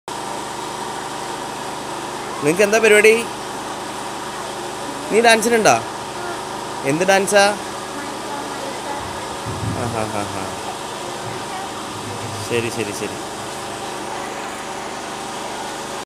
meenakshy,ayana prasad and aparna prasad kombnad memmory of school life and annual day
memmory of school life and annual day, ayana and aparna kombanad march 2014 krariyely lp school , kombanad, perumbavoor, kochi, ayana aparna and meenakshy